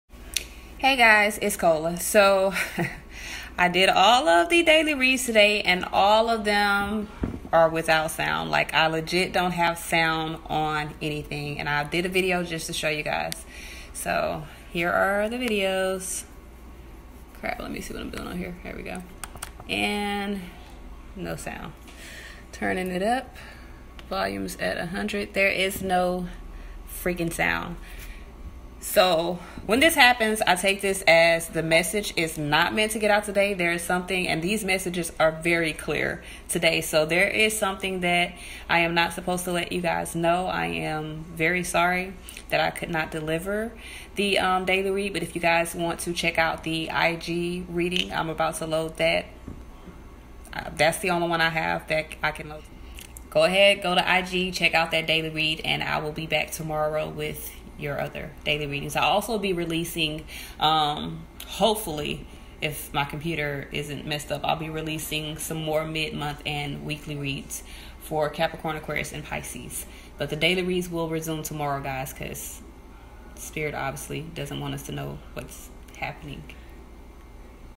October 9 Daily Reading Update
My apologies guys I am unable to load any videos today 😢